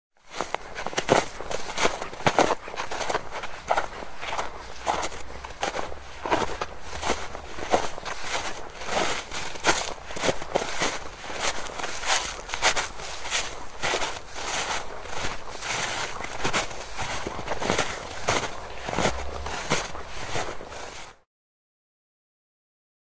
Crunching Snow Underfoot Sound Effect | Human Sounds
Walking on snow SFX effect. Sound of snow crunching. Sound of crunching snow underfoot. Download free sound effects for your video clips, games, apps.